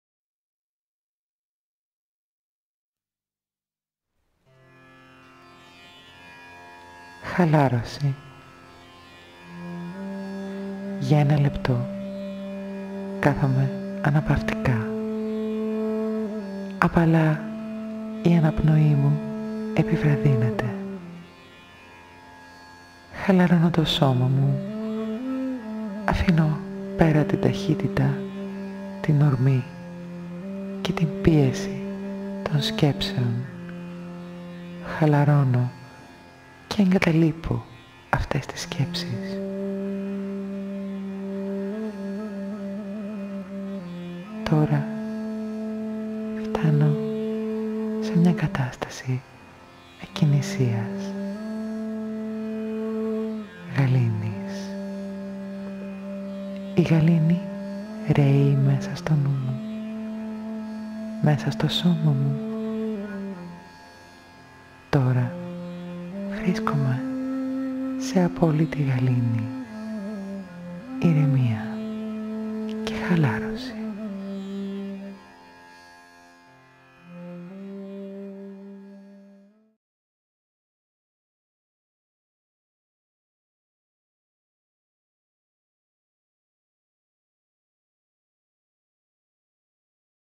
29. Χαλαρώνοντας - Διαλογισμοί του Ενός Λεπτού
~ Φαντάζεστε οτι ένα λεπτό θα ήταν αρκετό για να επαναφέρουμε τον εαυτό μας πίσω στη φυσική κατάσταση της εσωτερικής γαλήνης και της ευημερίας;

Οι "Διαλογισμοί του Ενός Λεπτού" θα σας βοηθήσουν να χαλαρώνετε, να επανεστιάζετε και να αναζωογονείστε σε μόλις ένα λεπτό.  Μπορείτε να γίνετε μία ισχυρή θετική δύναμη στη δική σας ζωή.  Δώστε στον εαυτό σας μόνο ένα λεπτό για να έχετε αυτή την εμπειρία τώρα.

~ Χαλάρωση - Παρουσία - Δύναμη - Έμπνευση - Αλήθεια ~